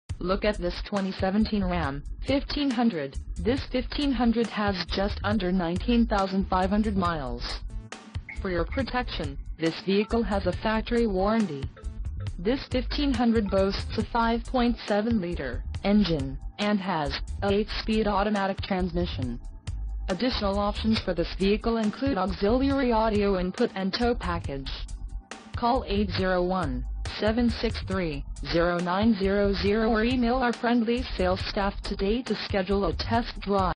2017 Ram 1500 SLT Truck - American Fork, UT
Watts Automotive, American Fork, UT - 2017 DODGE 1500 QUAD CAB SHORTBED SLT 5.7L HEMI 4X4 NEW LIFT